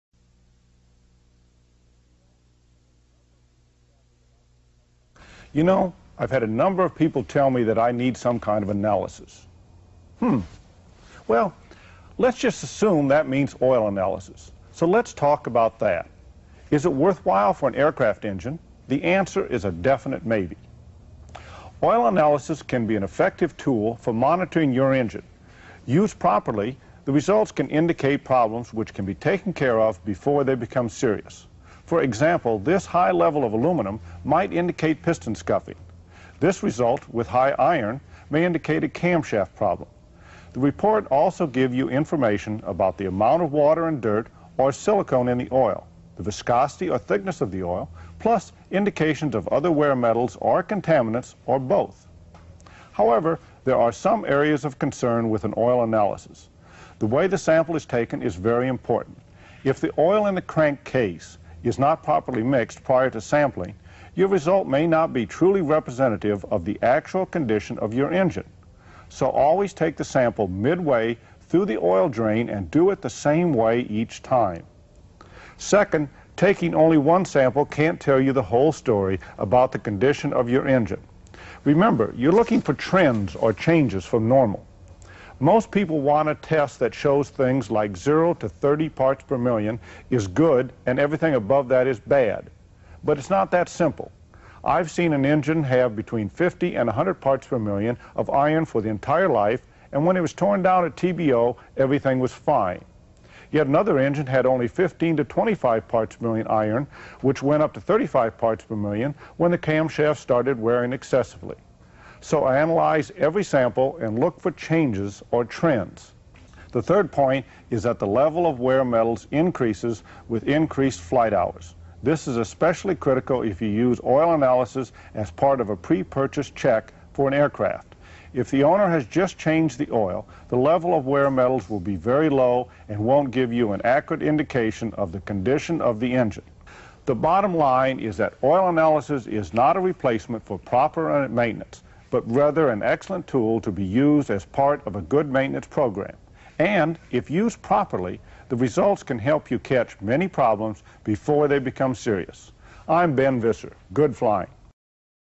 AeroShell Answers Oil Analysis - Is it right for you?
You know, I’ve had a number of people tell me that I need some kind of analysis. Hmm, well, let’s just assume that means oil analysis. So let’s talk about that. Is it worthwhile for an aircraft engine? The answer is a definite maybe. Oil analysis can be an effective tool for monitoring your engine. Used properly, the results can indicate problems which can be taken care of before they become serious. For example, this high level of aluminium might indicate piston scuffing. This result with high iron may indicate a cam shaft problem. The report also gave you information about the amount of water and dirt or silicone in the oil, the viscosity or thickness of the oil plus indications of other wear materials or contaminants or both. However, there are some areas of concern with an oil analysis. The way the sample is taken is very important. If the oil in the crank case is not properly mixed prior to sampling, your result may not be truly representative of the actual condition of your engine.

Most people want a test that shows things like 0 to 30 parts per million is good and everything above that is bad. But it’s not that simple. I’ve seen an engine have between 50 and 100 parts per million of iron for the entire life and when it was torn down at TPO everything was fine. Yet another engine had only 15 to 25 parts per million iron, which went up to 35 parts per million when the cam shaft started wearing excessively. So, analyse every sample and look for changes or trends.